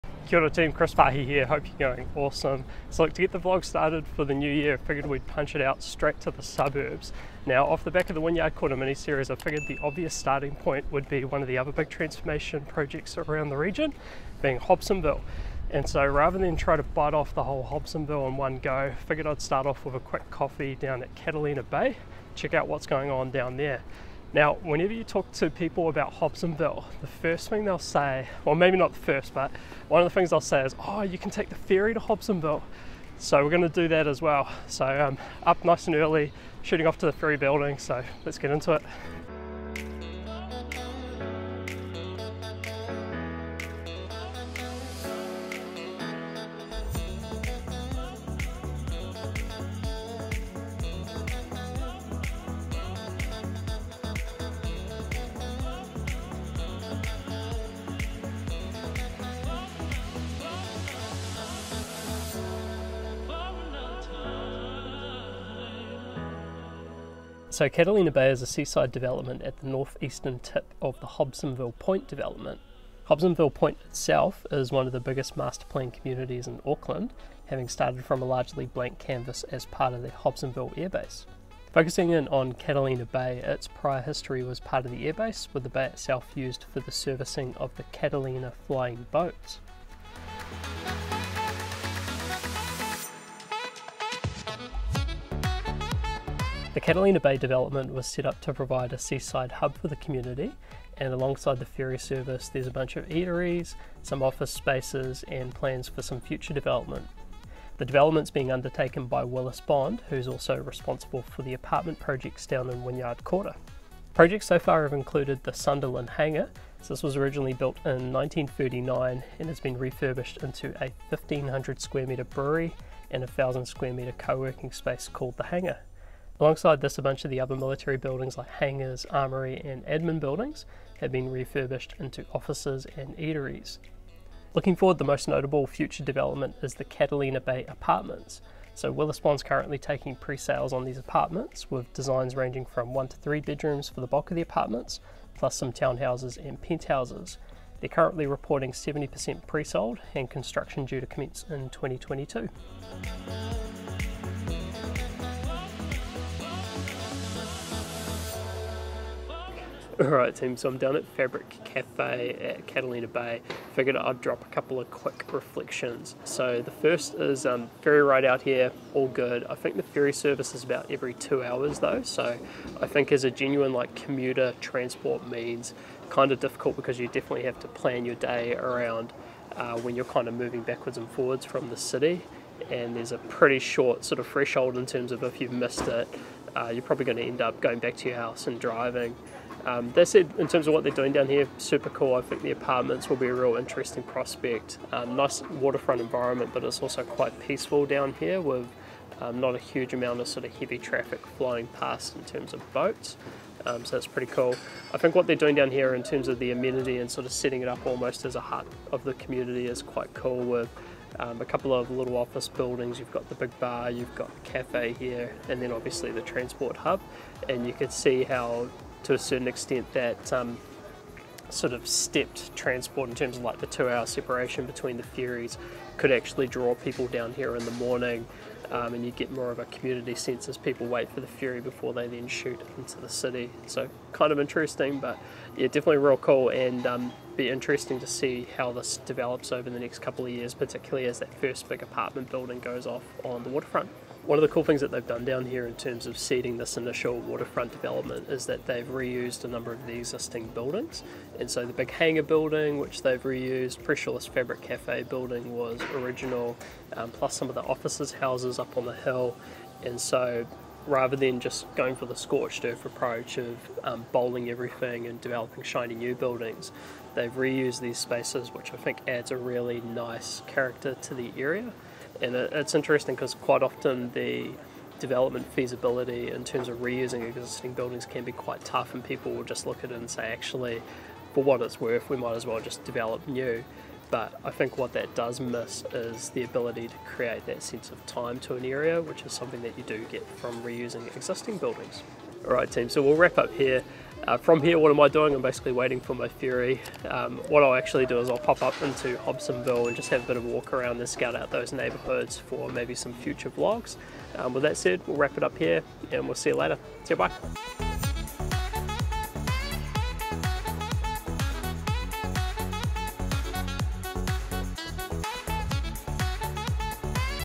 NZ real estate consultant breaks down development at Auckland's Catalina Bay
This vlog looks at development in the Catalina Bay precinct of Hobsonville Point, Auckland.

Chris Farhi is a property expert based in Auckland, New Zealand.

Ripping and/or editing this video will result in drama.This vlog looks at the development of apartments and hotels in the area...